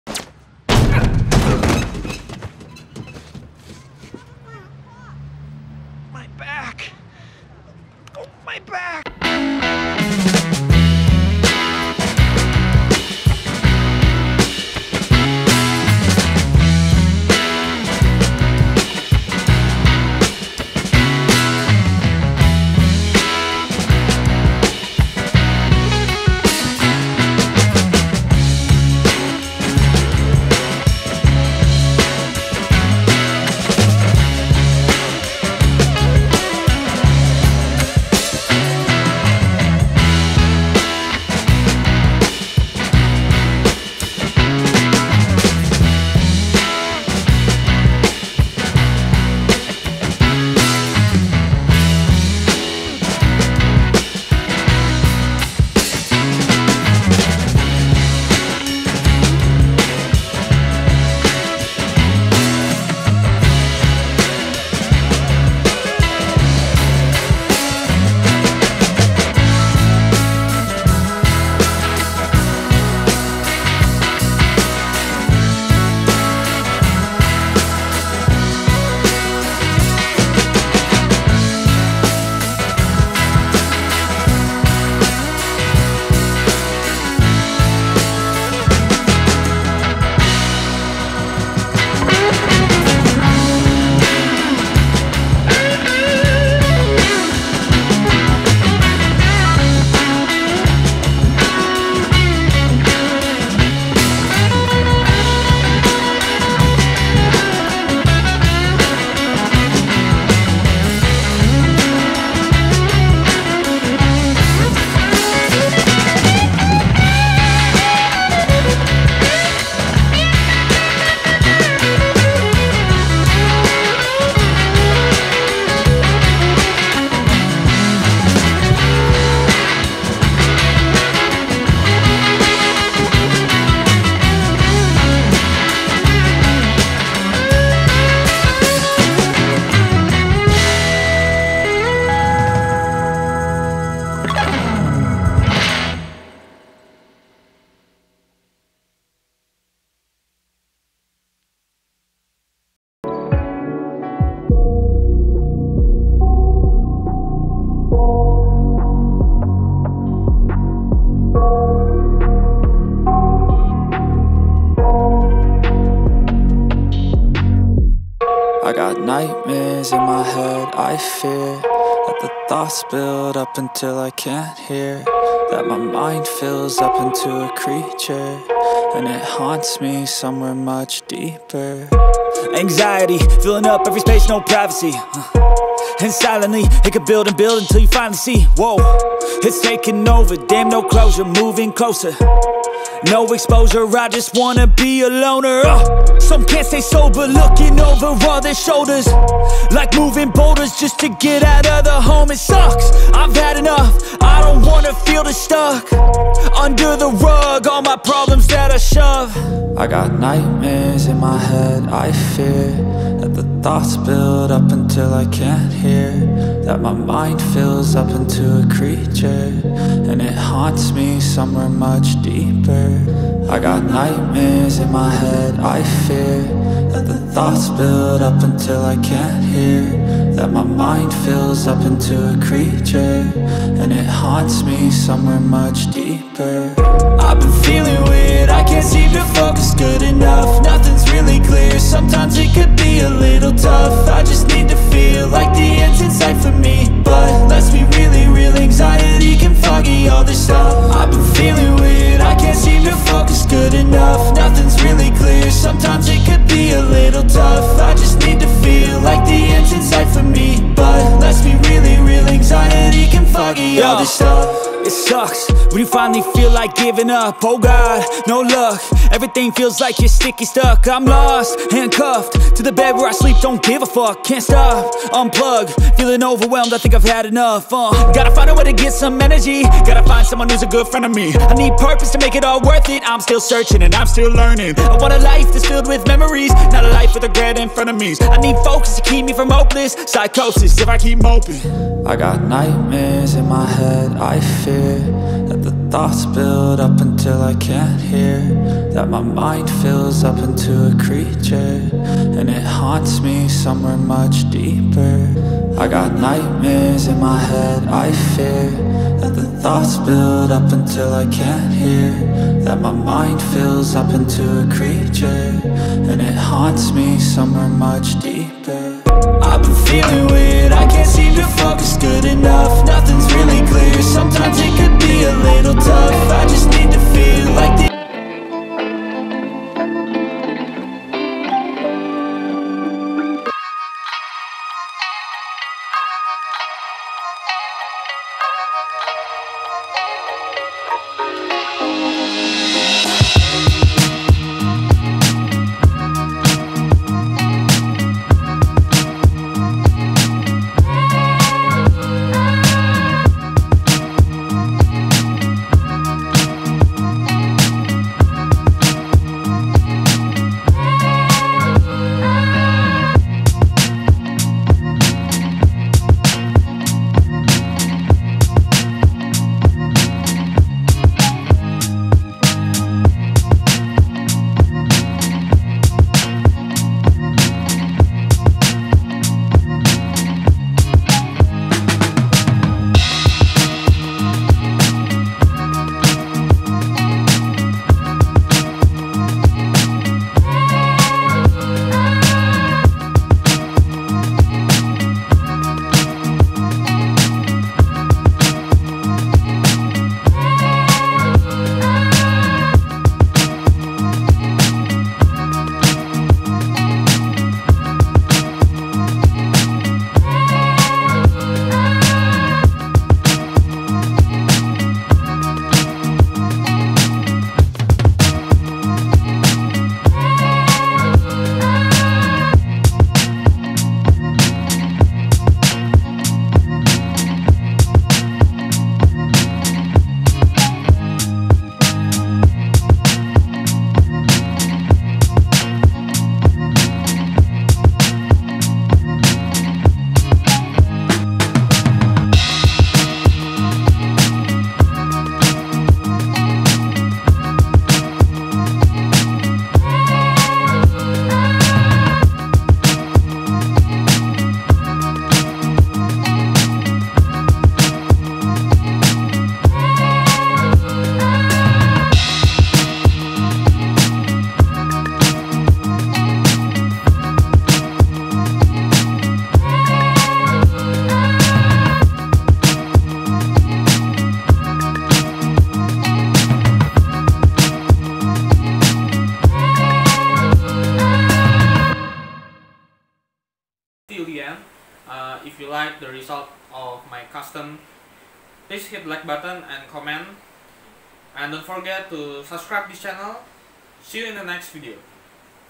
Painting Realistic Tobey Maguire Spider-Man 1/6 Scale - Spider-Man No Way Home
Custom by me,